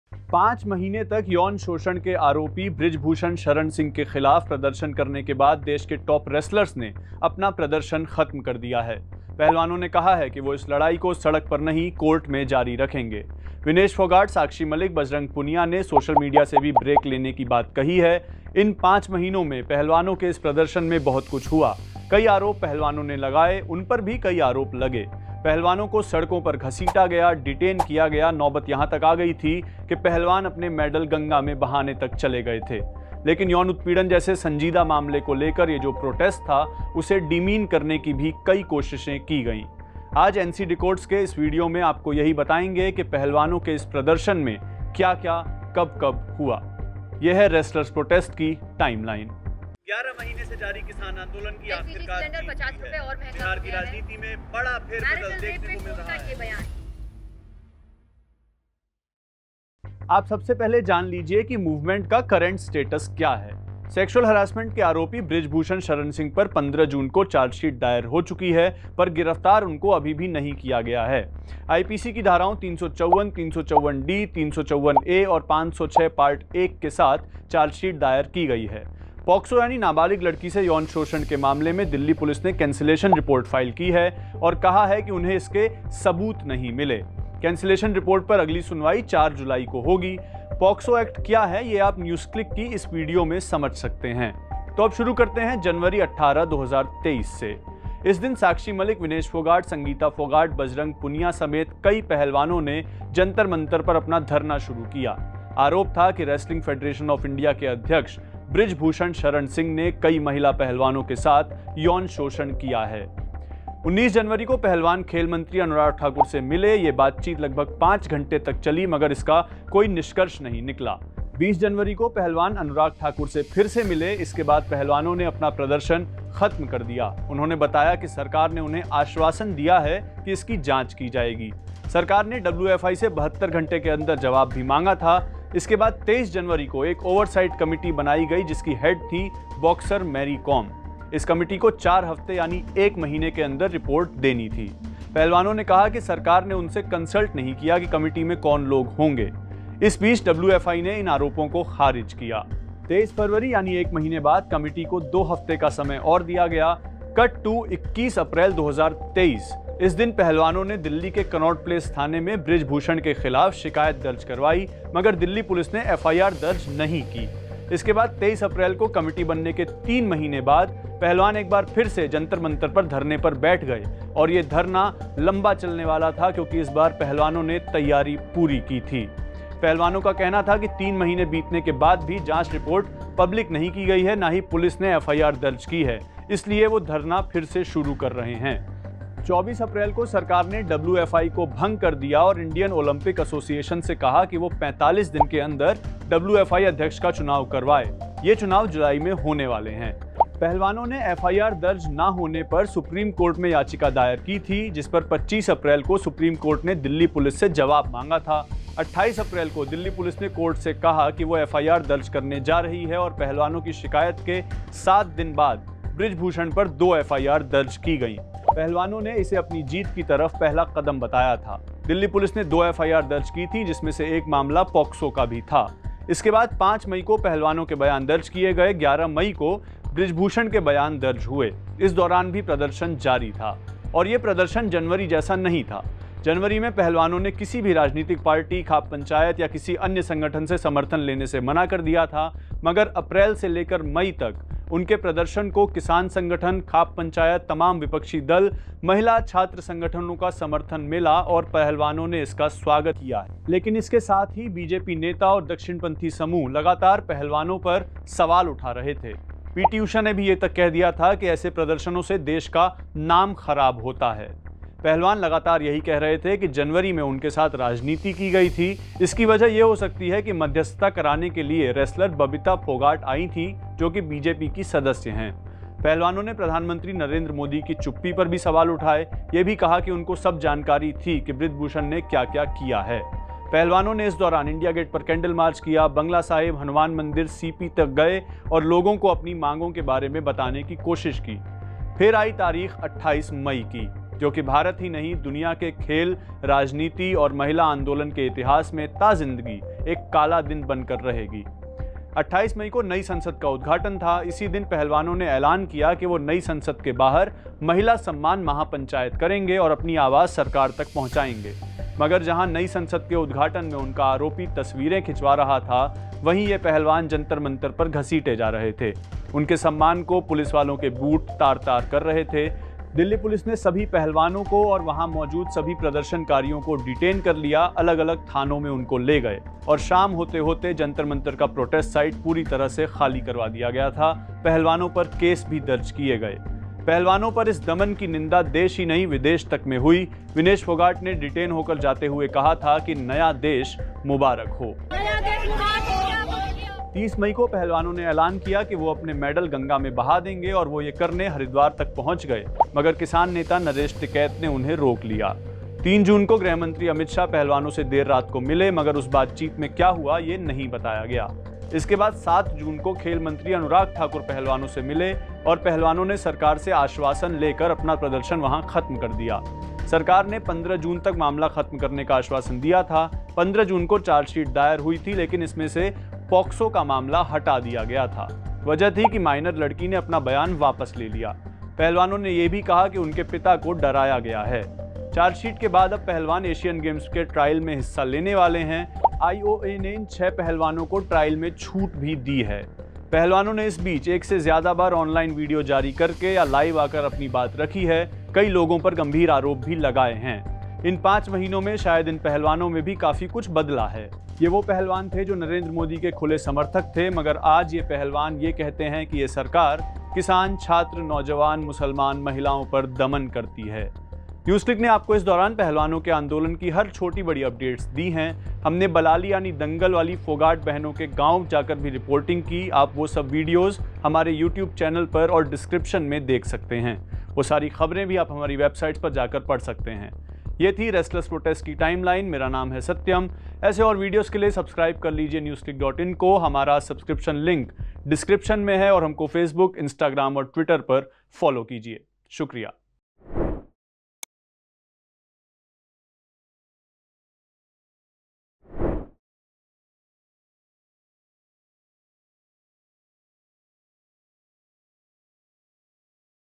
Wrestlers Protest Timeline : जानिए पहलवान आंदोलन में कब क्या हुआ?
Wrestlers Protest Called Off : 5 महीने तक यौन शोषण के आरोपी Brijbhushan Sharan Singh के ख़िलाफ़ प्रदर्शन करने के बाद देश के टॉप Wrestlers ने अपना प्रदर्शन ख़त्म कर दिया है। पहलवानों ने कहा है कि वो इस लड़ाई को सड़क पर नहीं कोर्ट में जारी रखेंगे। Vinesh Phogat, Sakshee Malik, Bajrang Punia ने सोशल मीडिया से भी ब्रेक लेने की बात कही है। इन 5 महीनों में पहलवानों के इस प्रदर्शन में बहुत कुछ हुआ, कई आरोप पहलवानों ने लगाये, उन पर भी कई आरोप लगे।

आज NCDecodes के इस वीडियो में आपको यही बताएँगे कि पहलवानों के इस प्रदर्शन में कब क्या हुआ? जानिए Wrestlers Protest की Timeline.